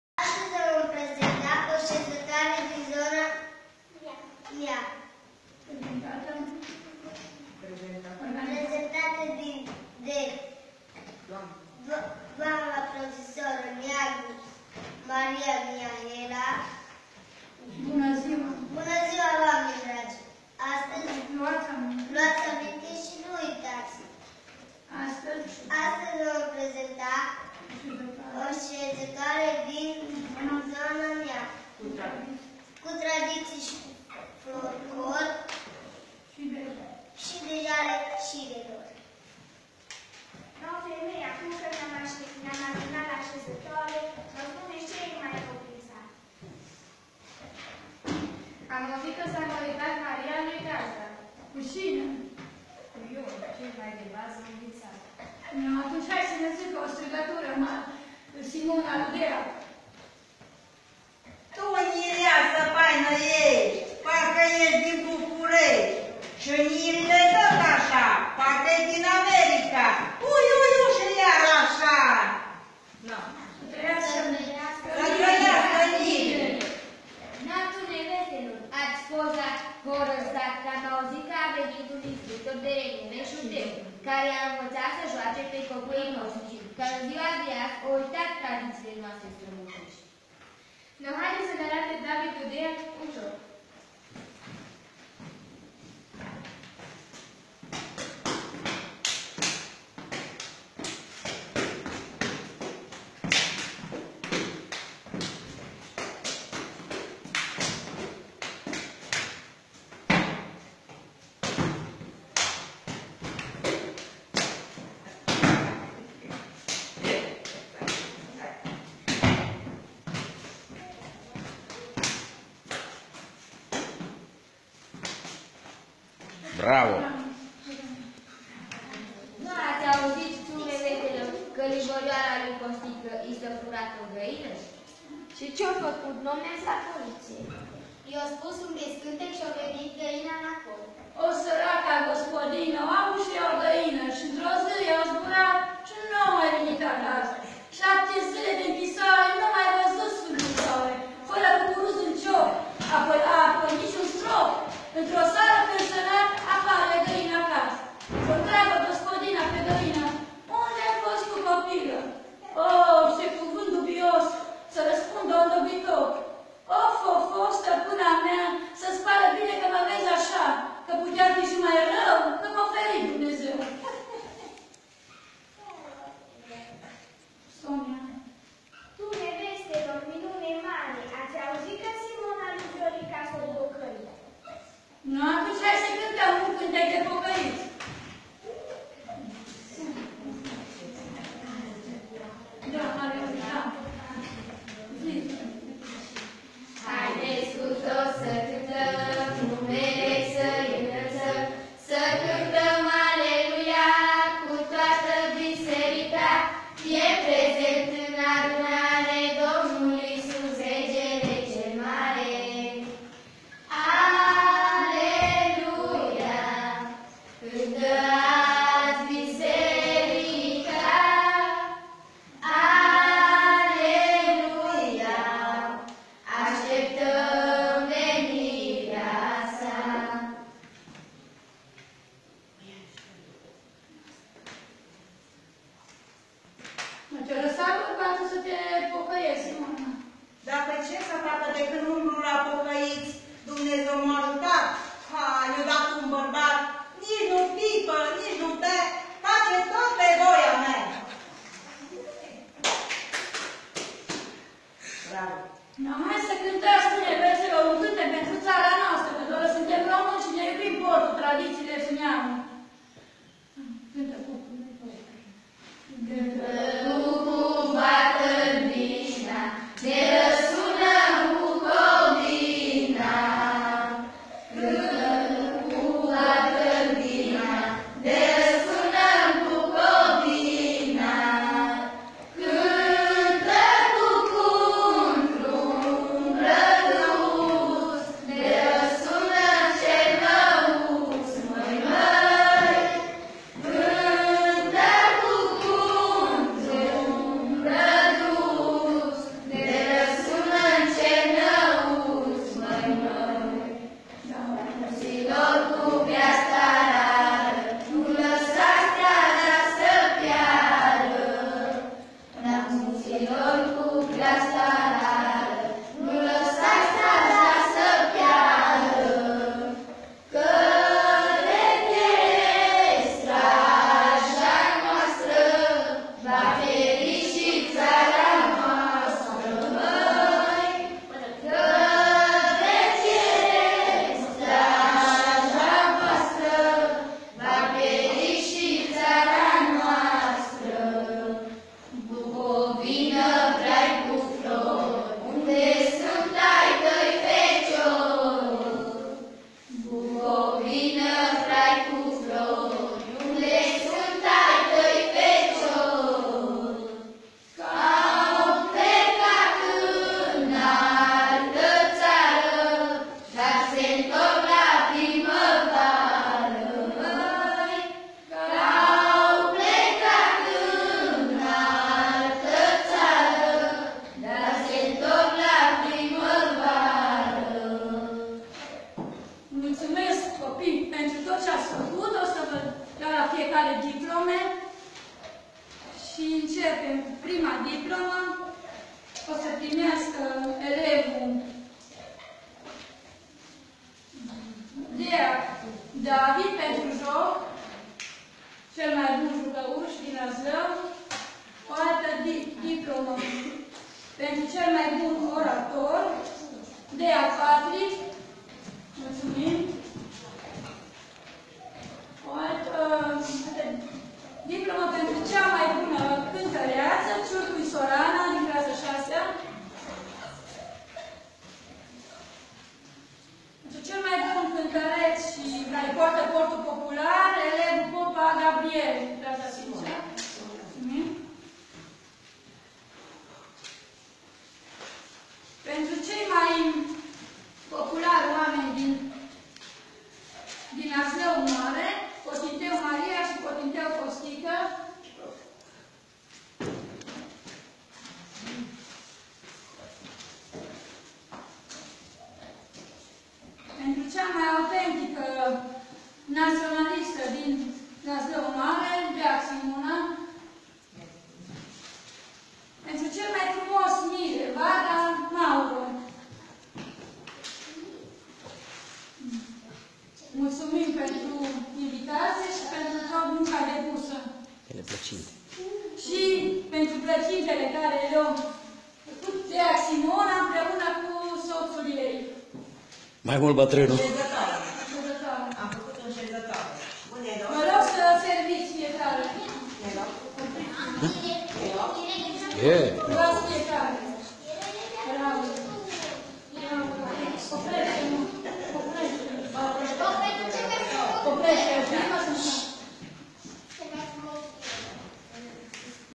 Tradiții şi folclor de la Şcoală Gimnazială ''Sfântu Nicolae'' din Laslău Mare din Săptămâna Altfel.
Filmat prin: Florin Deac.

Reactie din cineva din sat Laslau Mare: " Dragi laslaieni, din inregistrarea respectiva niste persoane prost intentionate si care nici macar nu sunt fii ai satului au incercat sa denigreze acest frumos loc, spunand ca in Laslau se fura, ca doar daca esti pocait ai familie buna (respect pentru unele confensiuni) dar laslaienii sunt oameni buni, harnici si fac cinste acolo unde merg, au fost, sunt si vor ramane ortodoxi. Cu respect in numele mai multor fii ai frumosului sat Laslau" [Trif Gabriela]

Every village has it own traditions — at this so called "sezatoare", gathering of the villagers at the local school,  traditions should be past to the next generation. What is happening here is that a minority of neoprotestanst is forcing their religion as the only one, not the Roman Orthodox traditions.